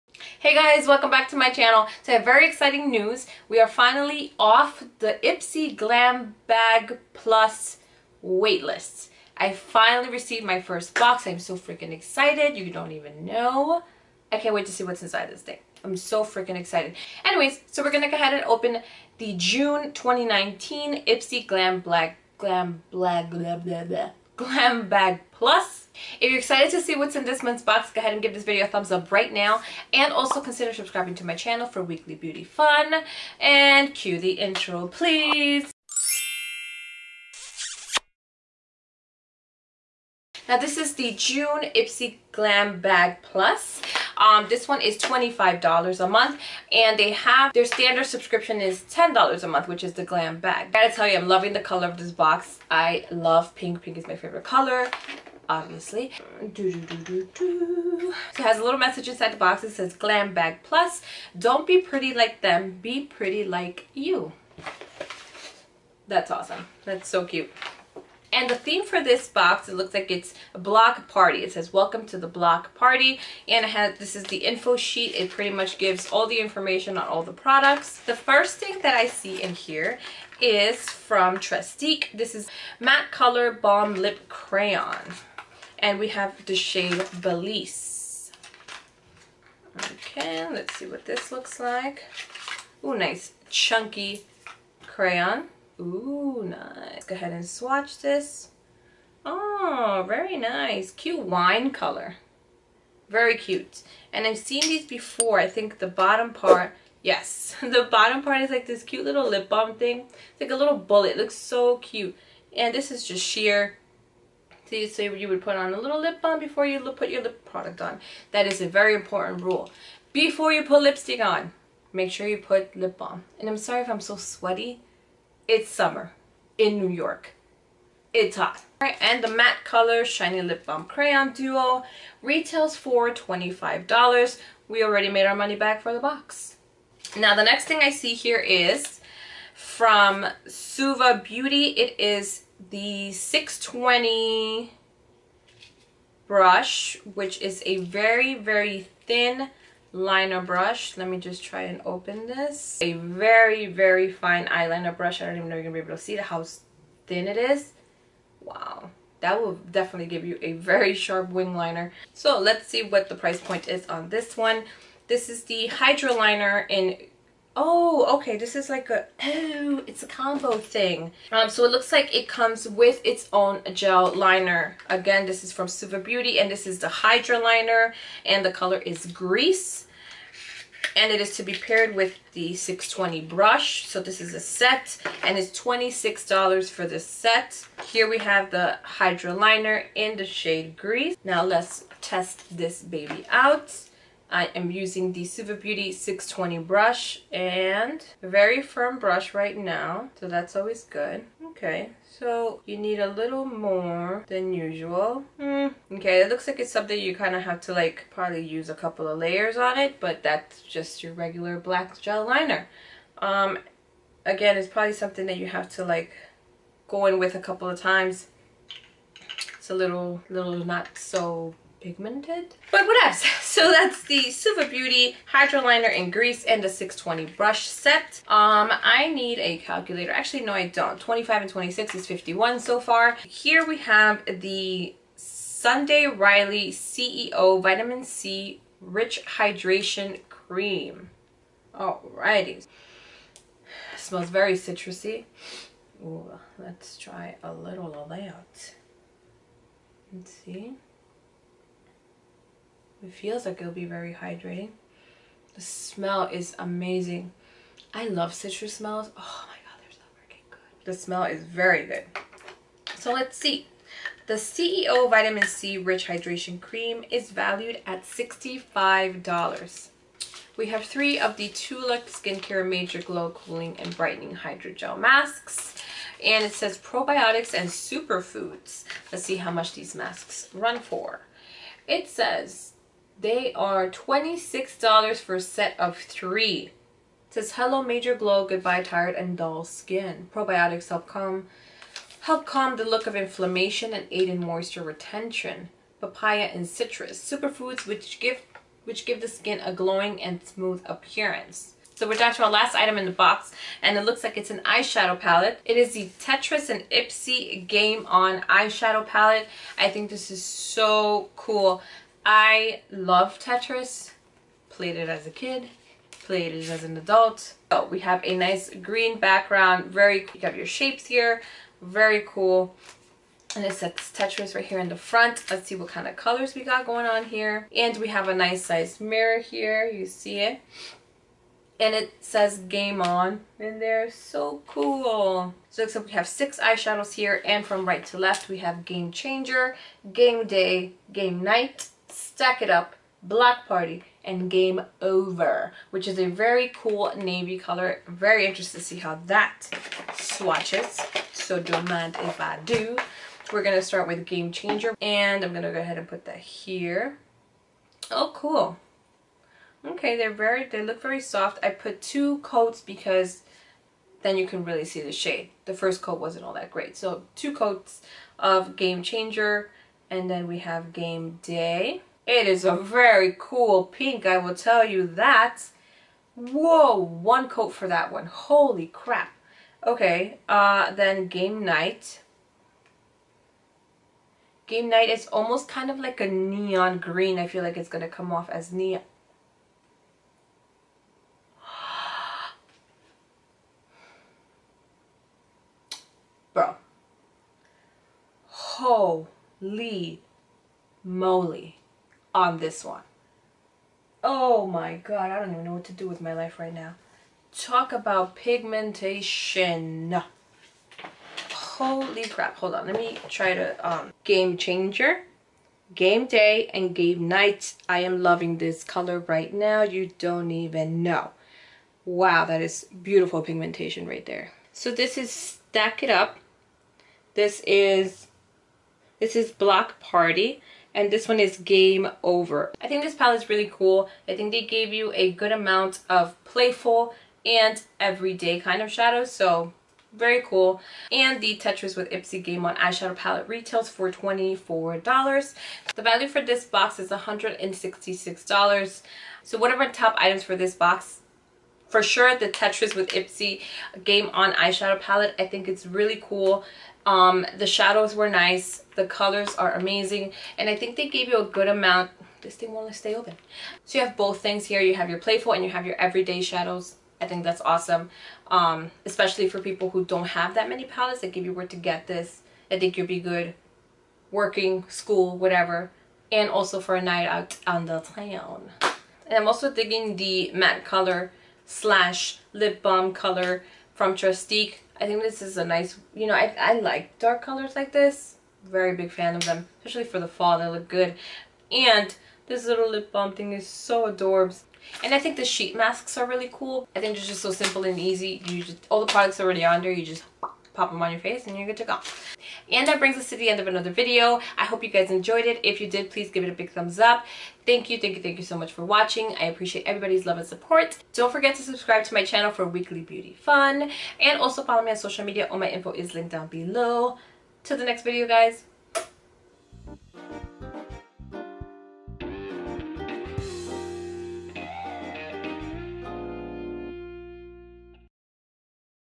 Ipsy Glam Bag PLUS Unboxing | June 2019 | MakeupPixie
So there I am relaxing one day and I get an email from IPSY that I am officially off the waiting list and my first "Glam Bag Plus" was on its way! So here we are unboxing it together. I hope you guys enjoy thanks again for clicking on this video I appreciate all the love and support.

Let’s be amigos! 🤗